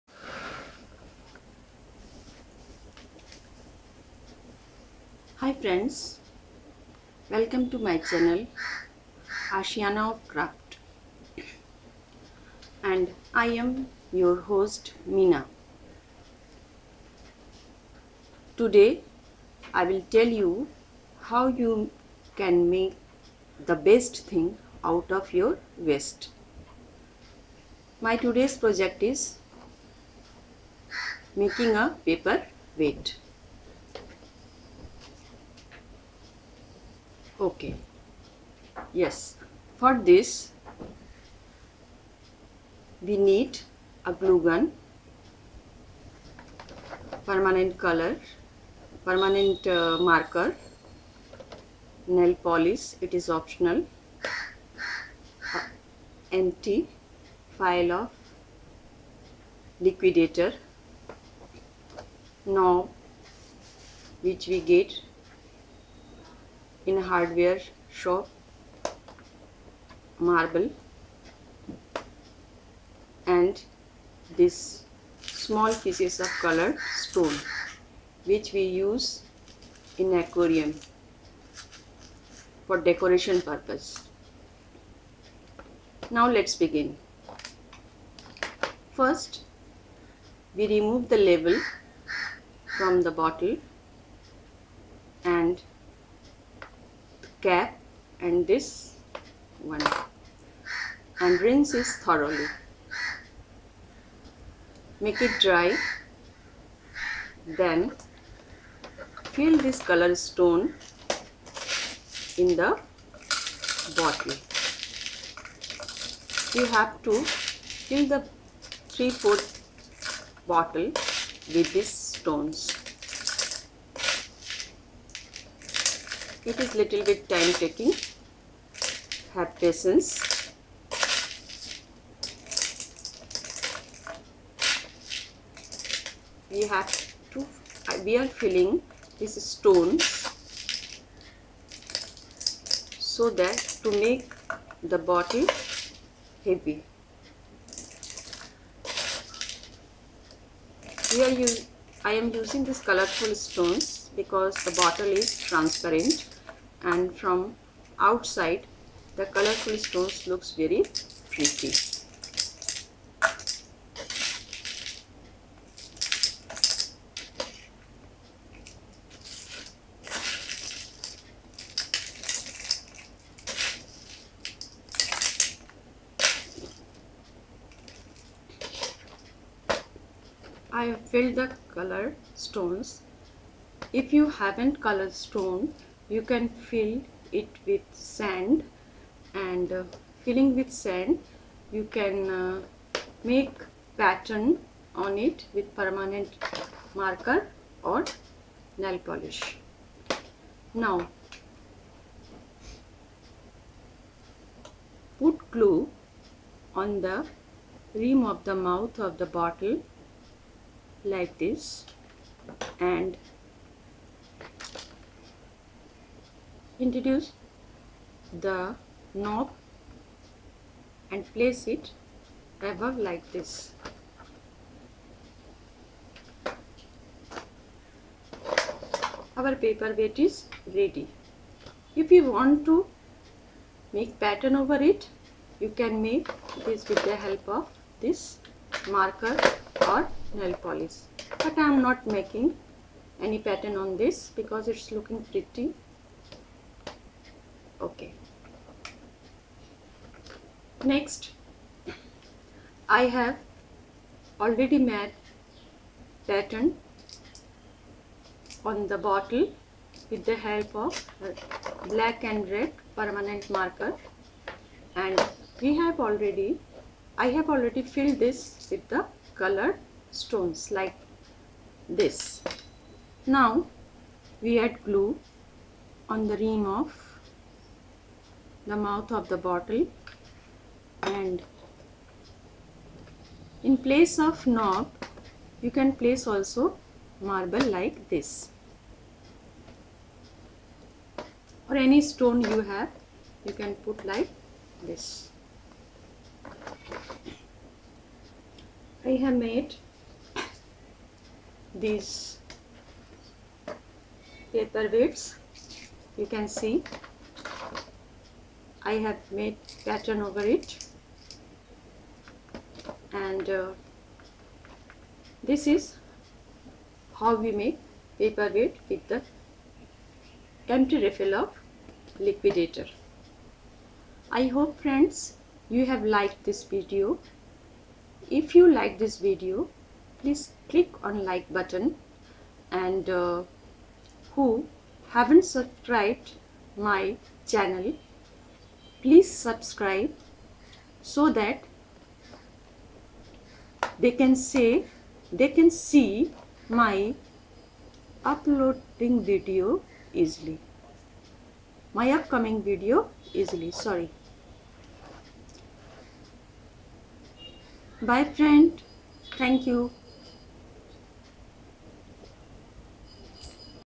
best out of waste...
Reuse of waste material quick and easy craft.recycling and reusing of empty bottle of mosquito repellent liquidator.and making paper weight